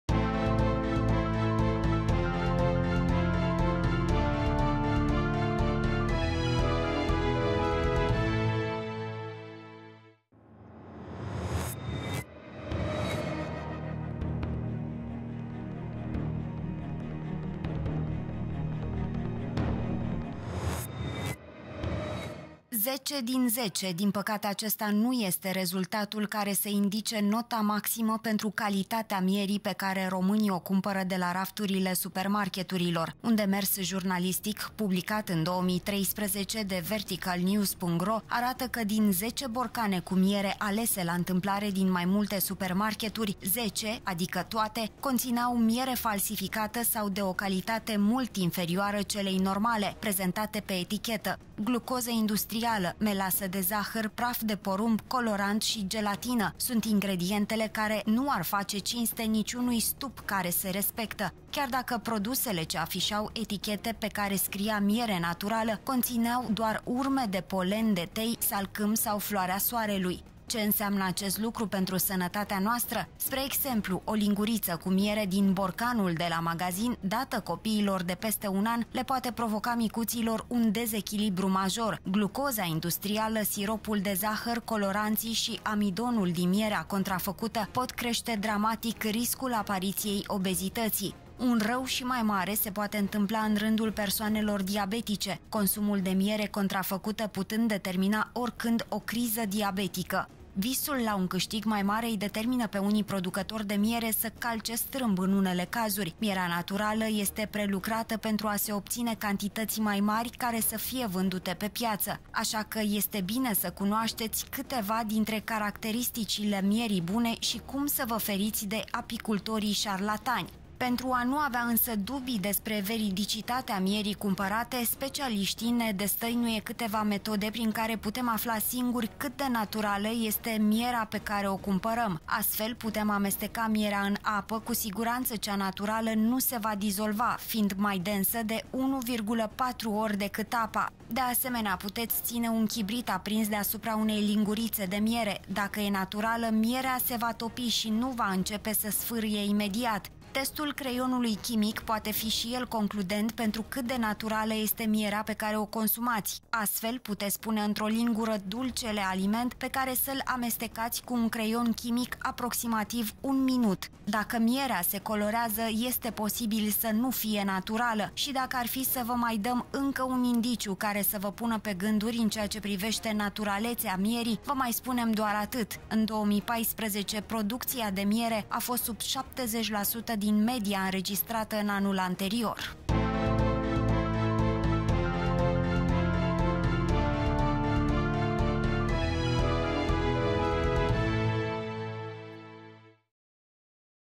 Stop minciunii alimentare
Până să fie descoperit zahărul din trestie şi apoi din sfeclă, mierea de albine a fost primul aliment dulce folosit de om, dintotdeauna fiind preţuită pentru numeroasele sale calităţi nutritive şi terapeutice.